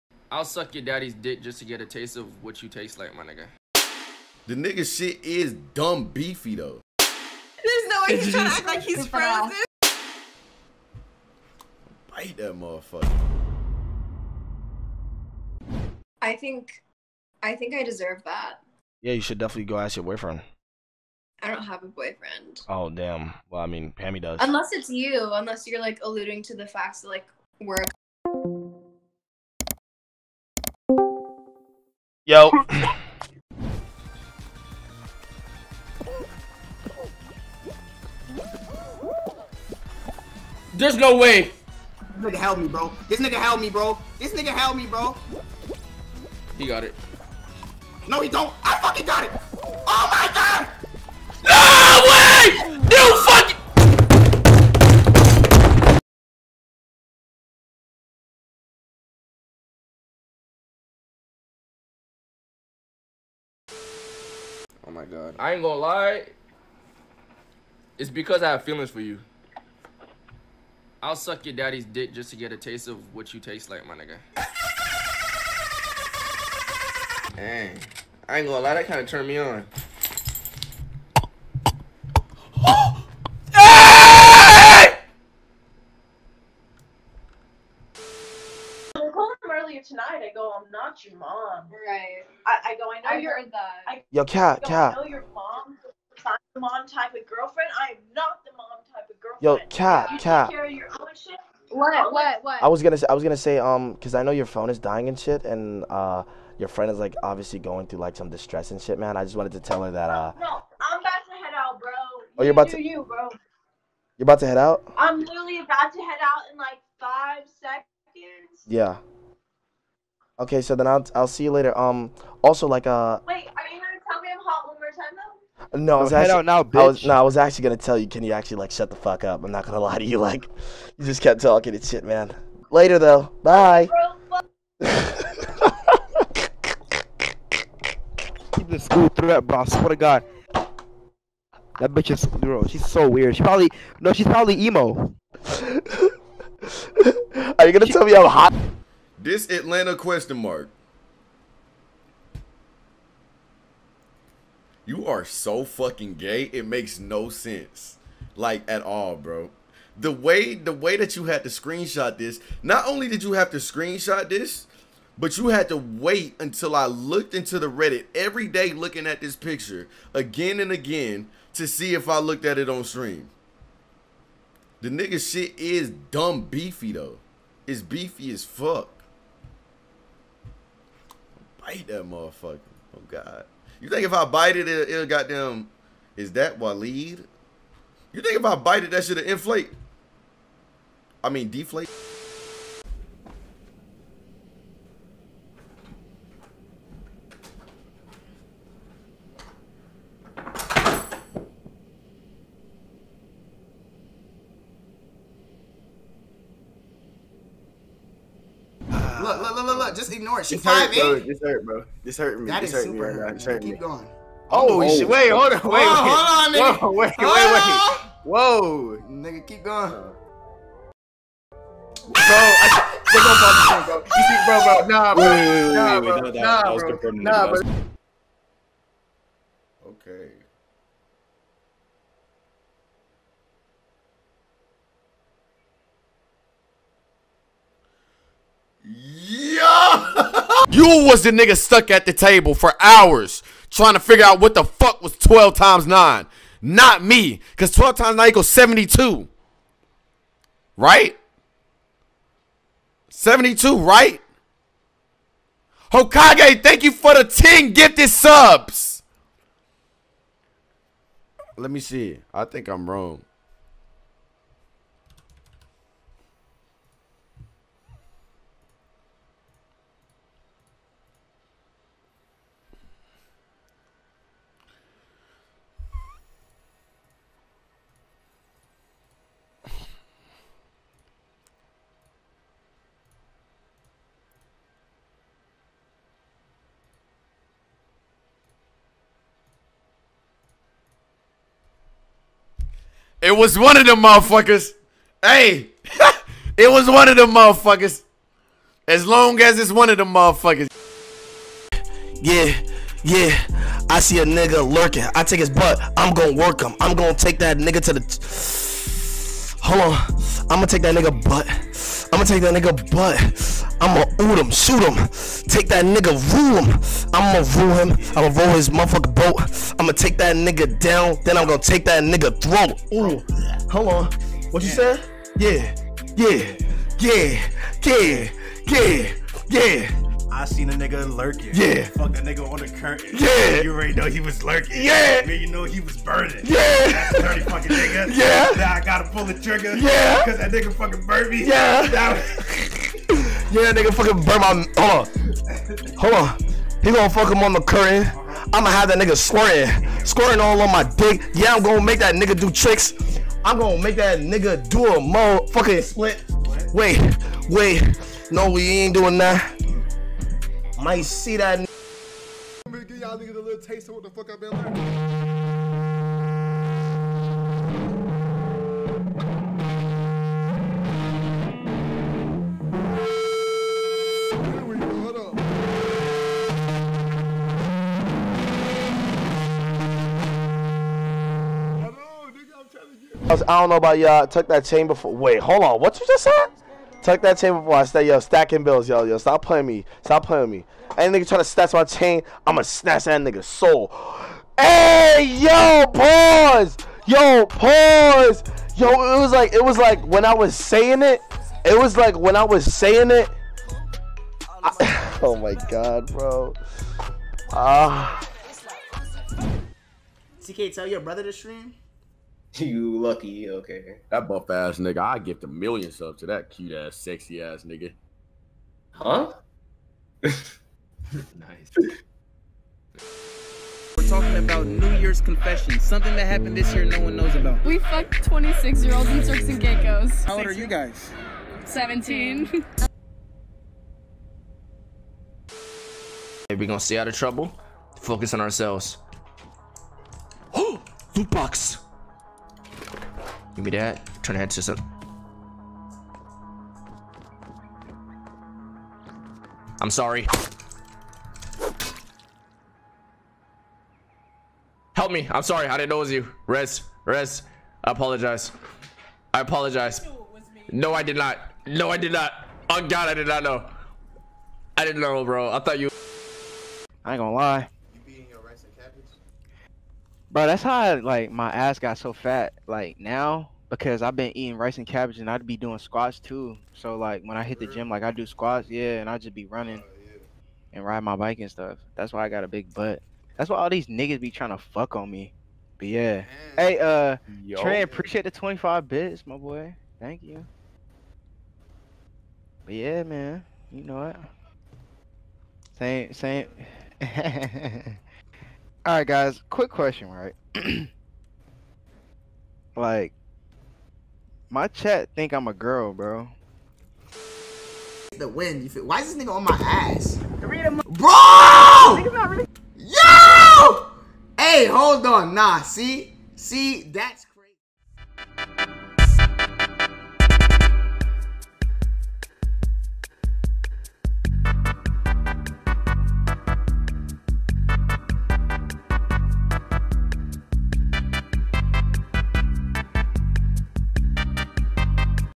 YourRAGE, BruceDropEmOff, Duke Dennis & MORE (TOP MOST FUNNIEST CLIPS)
Let me know what other content you would like me to make !!

Follow YourRAGE Gaming!

Follow Kai Cenat!

Follow BruceDropEmOff!

Follow Duke Dennis!

Follow Silky!

Follow ScumTK!